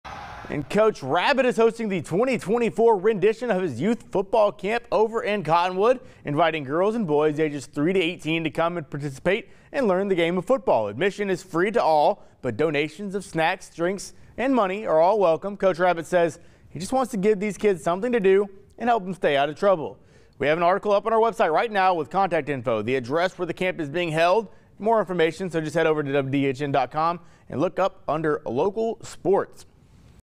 Coach Rabbit set to host 2024 Youth Football Camp in Cottonwood
Coach Rabbit will host his 2024 Youth Football Camp in Cottonwood, inviting boys and girls aged 3 to 18 to learn and participate in football.